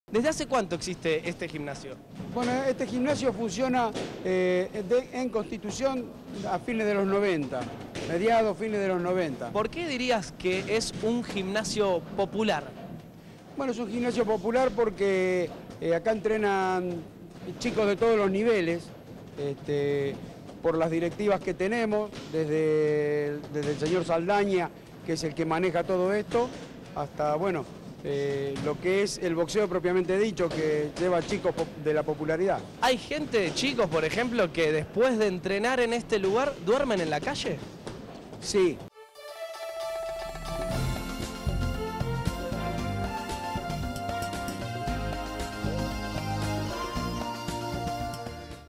Un gimnasio bajo la estación de Constitución - Telefe Noticias
Un gimnasio en un lugar especial, bajo una estación de tren. Entrevistamos a Alberto Santoro, profesor de boxeo, que nos cuenta más sobre este lugar en donde se practica y enseña boxeo. Conocelo.